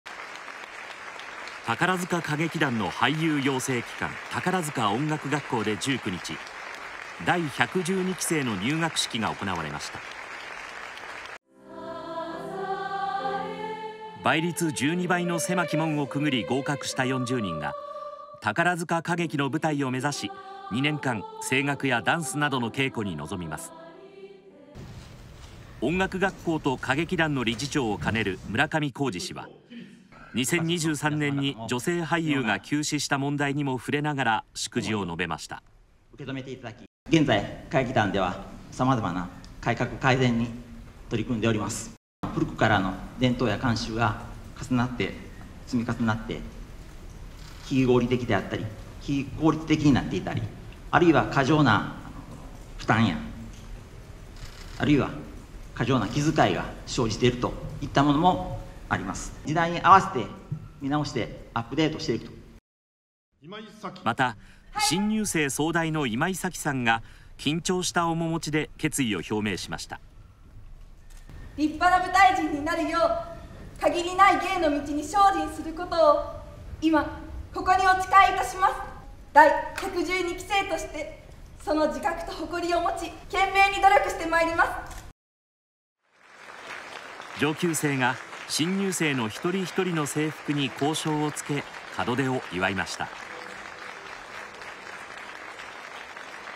タカラジェンヌ目指し「宝塚音楽学校」で入学式（2024年4月19日）
宝塚歌劇団の俳優養成機関「宝塚音楽学校」で19日、第112期生の入学式が行われました。倍率12倍の狭き門から合格した40人が宝塚歌劇の舞台を目指し、2年間、声楽やダンスなどの稽古に臨みます。音楽学校と歌劇団の理事長を兼ねる村上浩爾氏は、2023年に女性俳優が急死した問題にも触れながら祝辞を述べました。
村上理事長「現在歌劇団ではさまざまな改革改善に取り組んでいる。古くからの伝統や慣習が積み重なり、非合理的、非効率的になっていたり、過剰な負担や過剰な気遣いが生じているものもある。時代に合わせてアップデートする」
また、新入生総代の今井咲さんが、緊張した面持ちで決意を表明しました。
今井咲さん「立派な舞台人になるよう、限りない芸の道に精進することを誓う。第112期生として自覚と誇りを持ち、懸命に努力していく」
上級生が新入生の一人一人の制服に校章を着け、門出を祝いました。